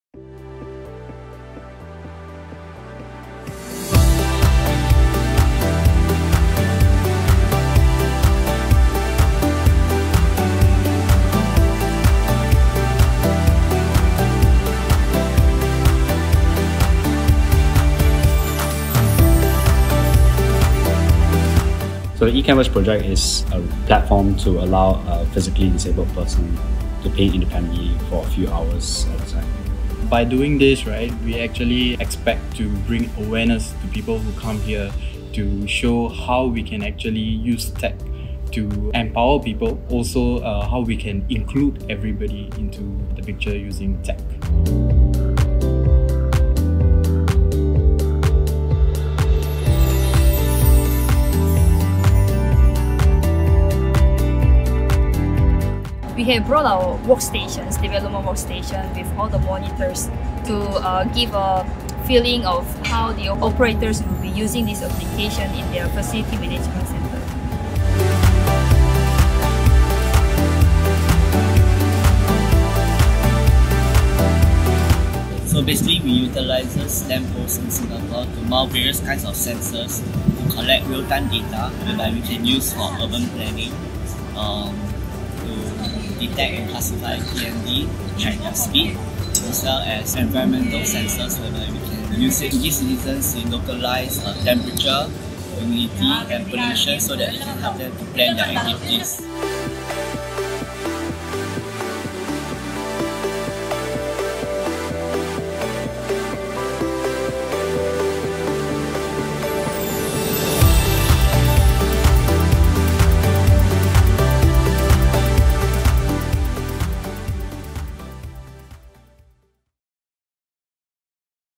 3 things we brought to Barcelona!
The Smart City Expo World Congress 2019 is a meeting point for the global smart city ecosystem, gathering top urban innovations and empowering cities to create a better future through promoting innovation, partnerships, and business opportunities.

This year, Singapore had the privilege of hosting our first national pavilion at the expo, showcasing 3 of our nation’s latest innovations towards a sustainable and inclusive future - the E-Canvas, Smart Sensing Estate Management System, and the Lamppost as a Platform.

It was an honour to contribute to the global smart city ecosystem and learn about other tech-driven solutions from smart cities across the globe.

Read more about what we did at the Smart City Expo World Convention 2019